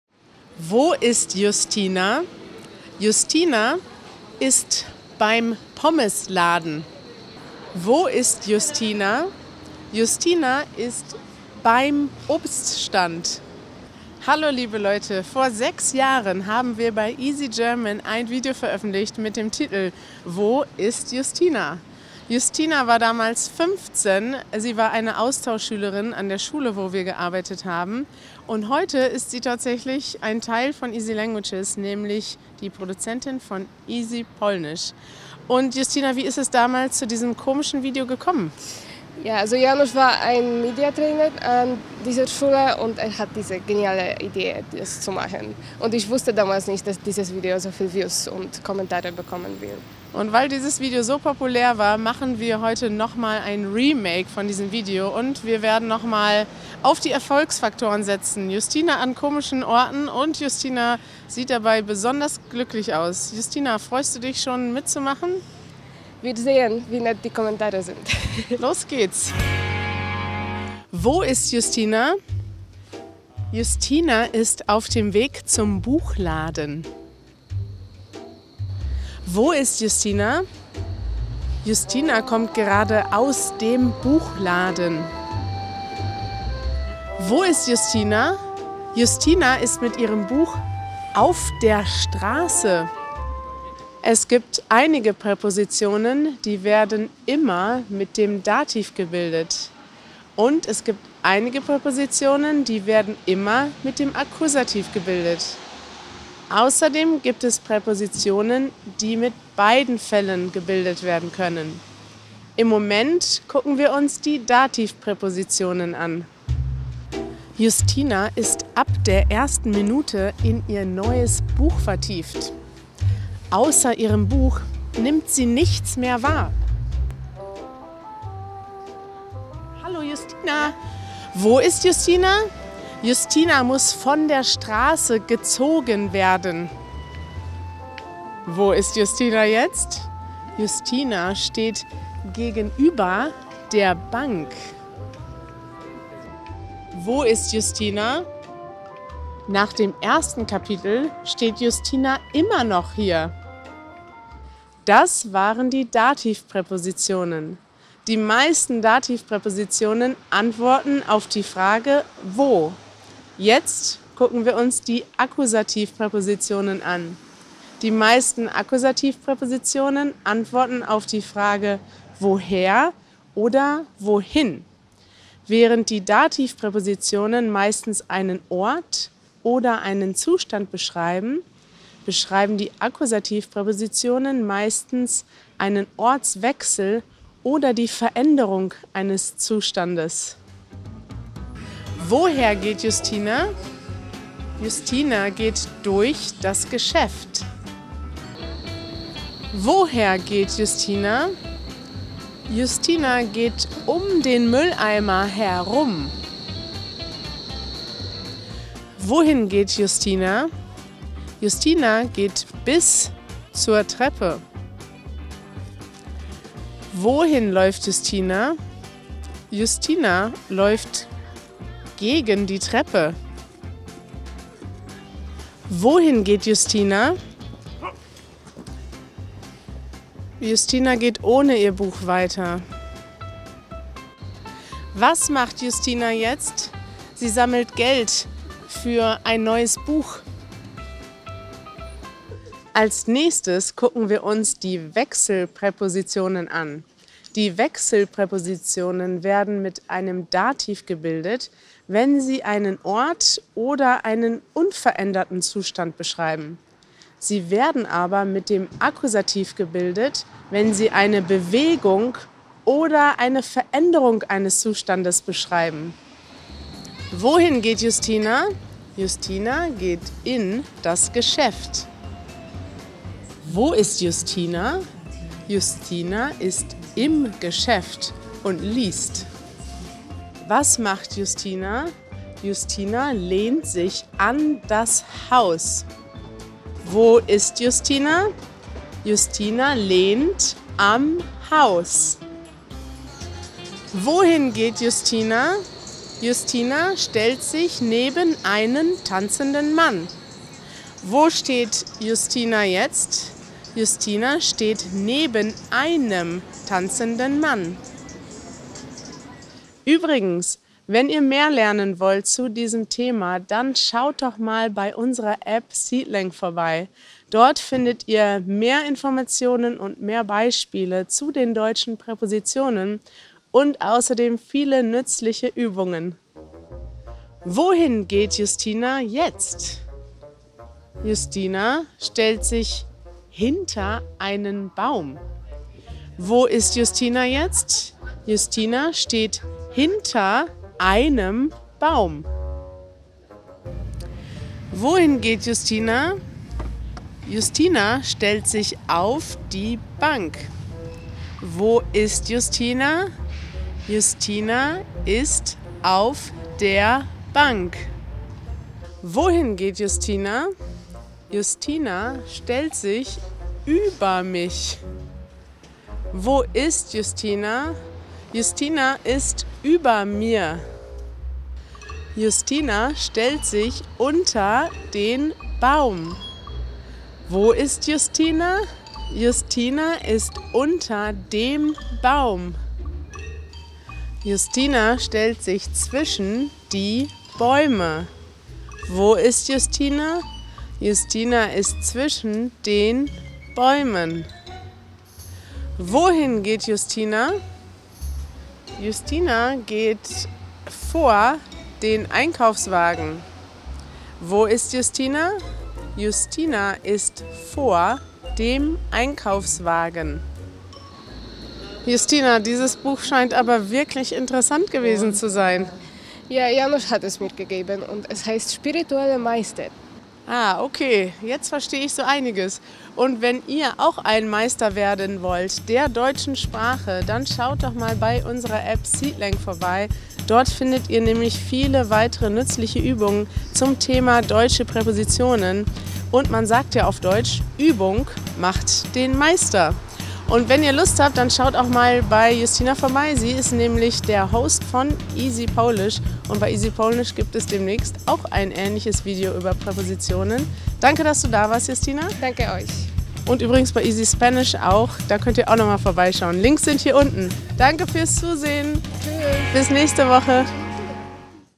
German Prepositions with Dative and Accusative (Wo ist Justyna?) | Super Easy German (120)
Hosts of this episode: Justyna Fuchs and Carina Schmid
Camera: Janusz Hamerski, Francisco A. Soza
Edit: Janusz Hamerski / Carina Schmid
Translation: Ben Eve